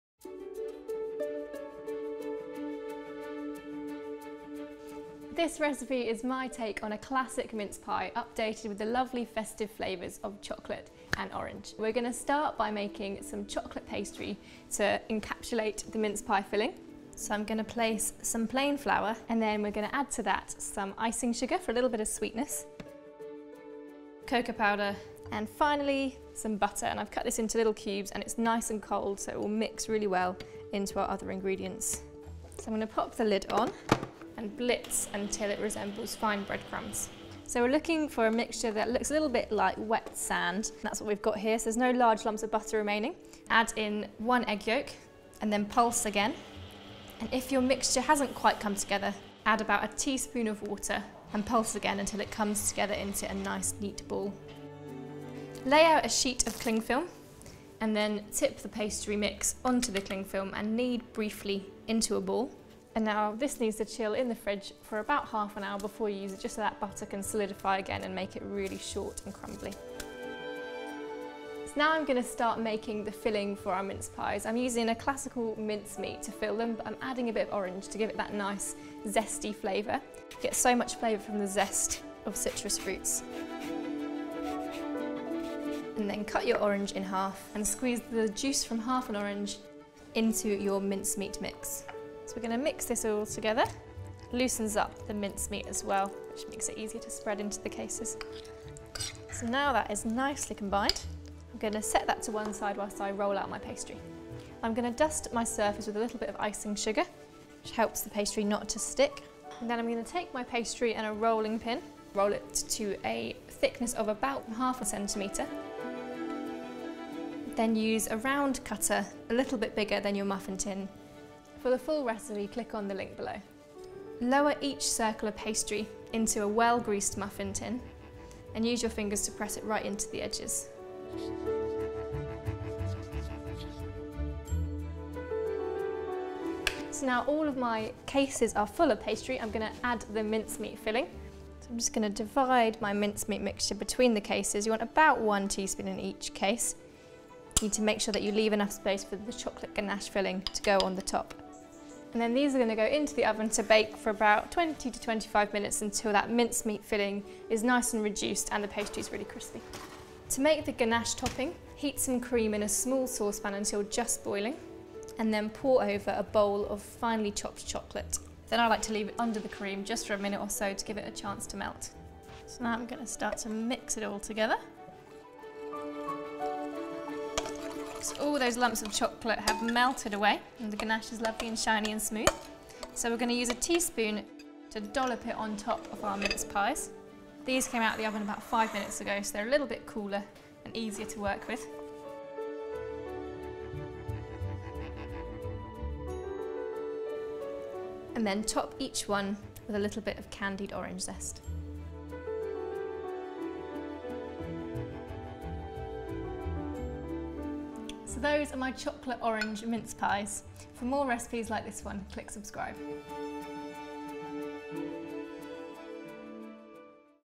Martha Collison's Chocolate Orange Mince Pies | Waitrose & Partners
Martha Collison makes one of the festive favourites but with a unique yet delightful twist.